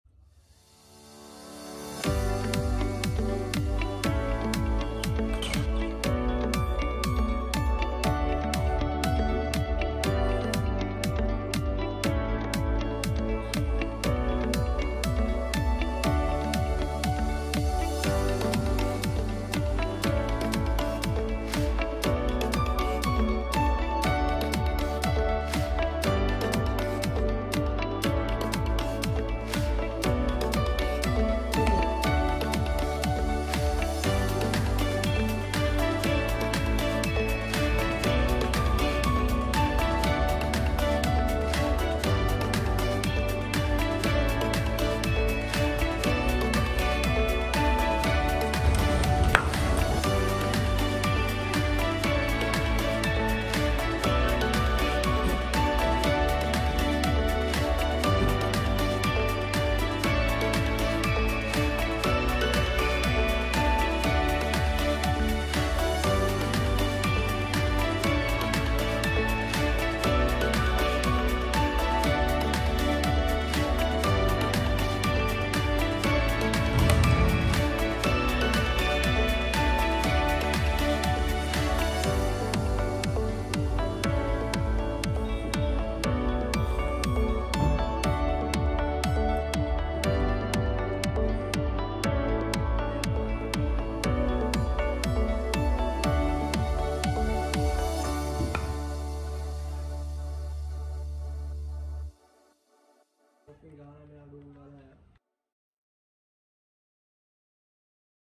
පොඩි සනා වගේ ගහන කොහොමද පාරවල් ටික | Batting like Kusal Janith Perera
Sri Lankan youngster, Lahiru Lakshitha hit the ball like Sanath Jayasuriya and Kusal Janith Perera.